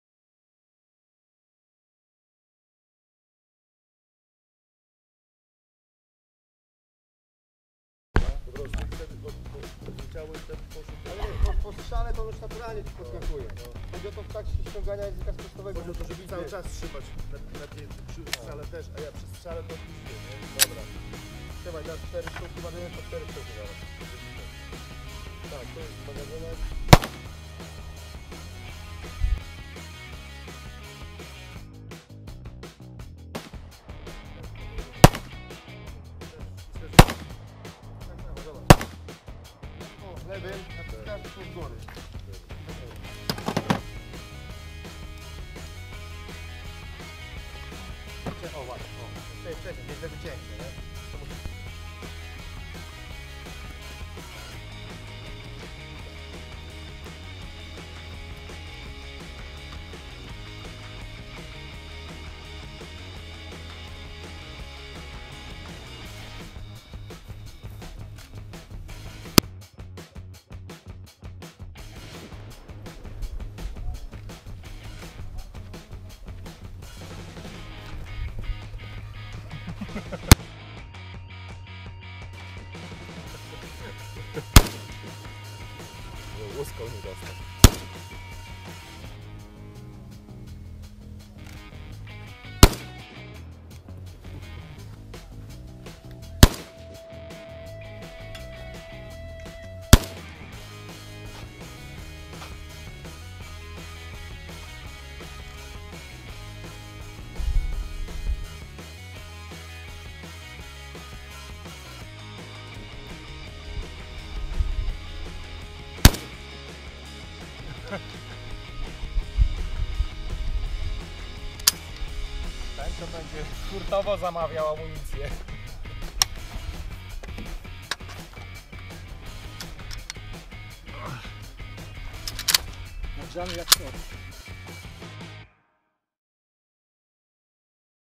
LEGION WSCHODNI - Radek - strzelanie z CZ i KAŁACHA
Kilka strzałów na strzelnicy w Turośni Kościelnej.

Pierwszy raz ze 100metrów z kałacha:)